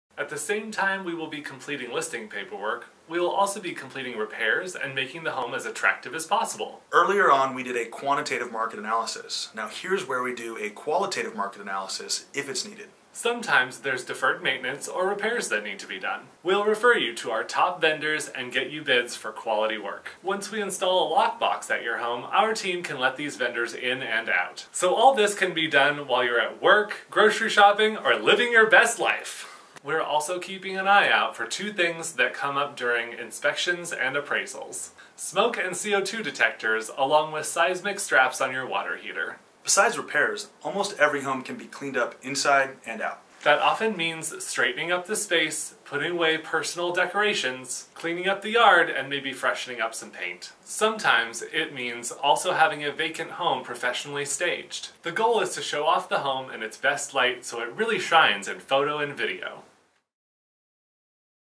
Selling Your Whatcom County Home | Part 4: Repairs & Curb Appeal
In this series, Leo, Chris and Tiffany introduce five key parts of getting ready to sell your home.

Meet the Cohen Group NW (a team under Muljat Group Realtors)